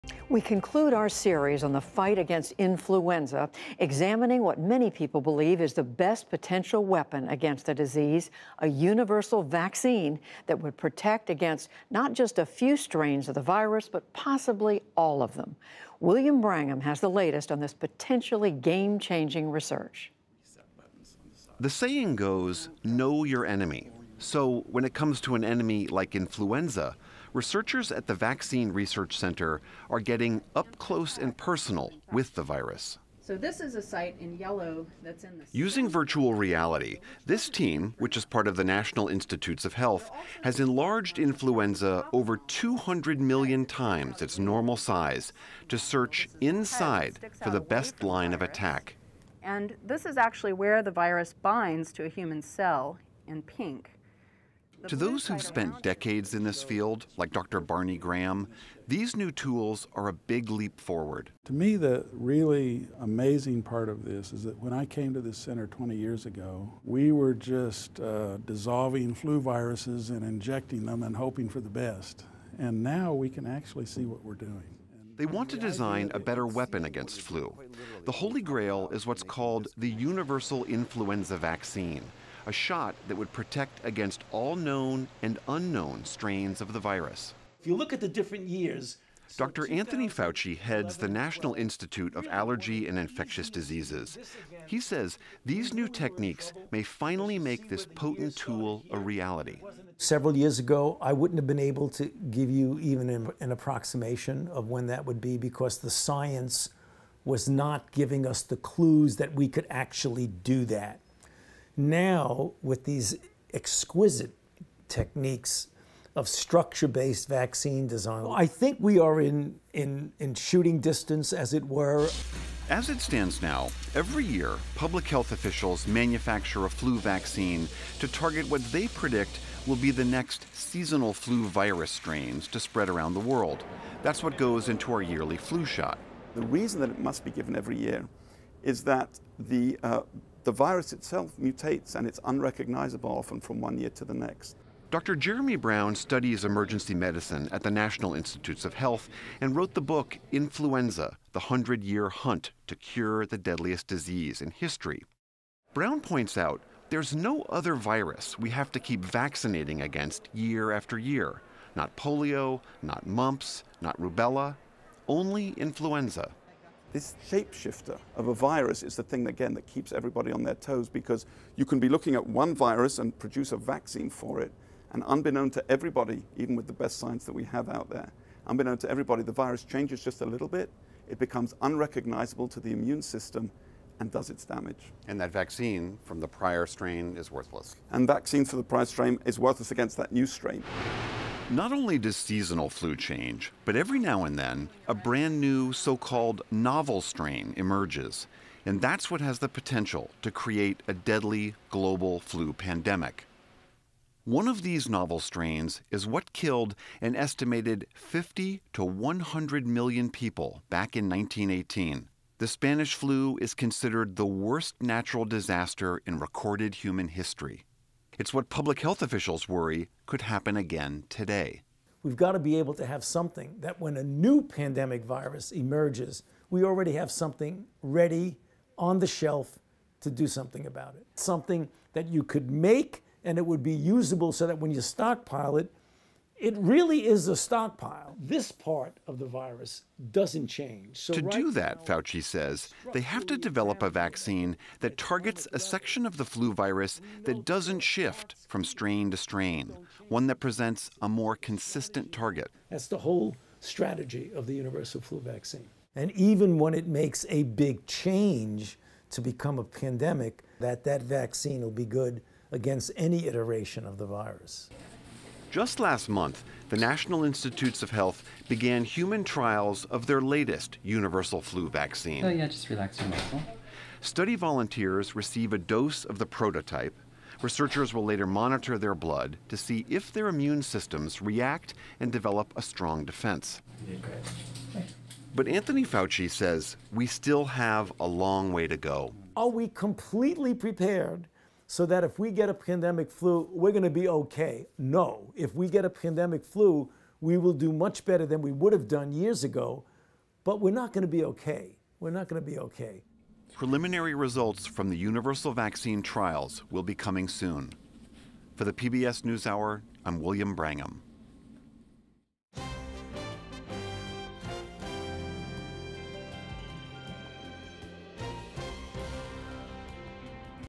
A universal flu vaccine could finally be within sight
Influenza is a shape-shifter virus that could spark a global pandemic. Researchers at the National Institutes of Health are working to deliver what is referred to as The Holy Grail in the fight: a universal flu vaccine that could protect against all strains of the virus. William Brangham concludes our pandemics series by learning what it will take to develop a universal vaccine.